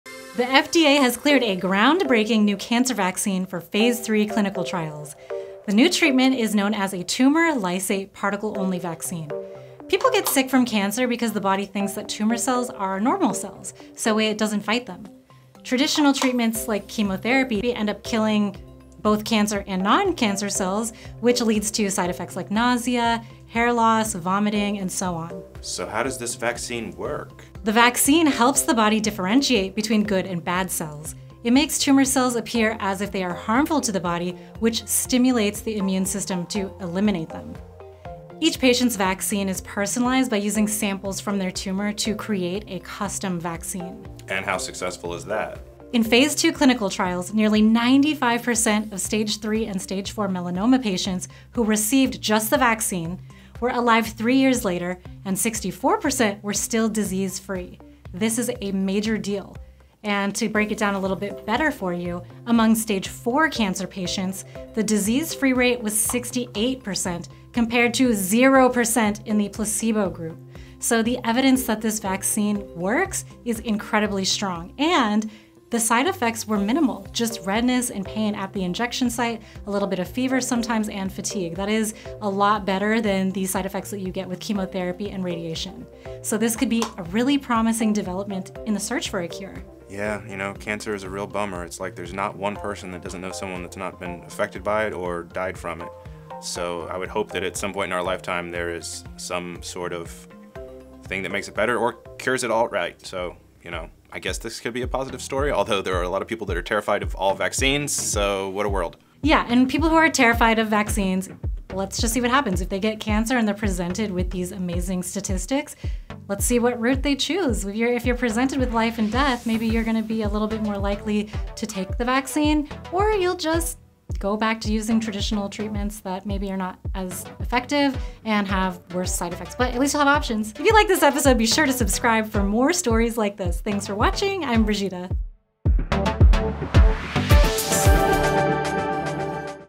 Breakthrough Personalized Cancer Vaccine: Hope for a Cure!
Get ready for a groundbreaking revelation in the fight against cancer! The FDA has given the green light for phase three clinical trials of a revolutionary Tumor Lysate Particle Only vaccine. In this must-watch video, journalist Brigida D’Agostino and director Tyson Wheeler Persall   unpack the science behind this game-changing treatment that's making waves in the medical world.

🩸 Traditional cancer treatments often come with harsh side effects, but this vaccine aims to revolutionize the approach. Learn how the Tumor Lysate Particle Only vaccine helps the body distinguish between good and bad cells, stimulating the immune system to target and eliminate harmful tumor cells. The best part? Each patient receives a personalized vaccine crafted from samples of their own tumor, promising a unique and effective treatment.

📈 The results from phase two clinical trials are nothing short of astonishing. Discover the impressive statistics, with nearly 95 percent of stage 3 and 4 melanoma patients who received the vaccine still alive three years later, and 64 percent remaining disease-free. Even among stage 4 cancer patients, the disease-free rate was a remarkable 68 percent, compared to zero percent in the placebo group. Minimal side effects, including redness, pain at the injection site, fever, and fatigue, hint at a potential breakthrough in the quest for a cure.

🌐 Join us as we explore this promising development, discuss its implications for the future of cancer treatment, and celebrate the hope it brings to patients and their families. Click play now and be part of the conversation! 💪🎗️

📝 Written and Produced by Brigida D’Agostino
🎬 Directed and Edited by Tyson Wheeler Persall @TysonXIX

Ep20 E Cancer Vaccine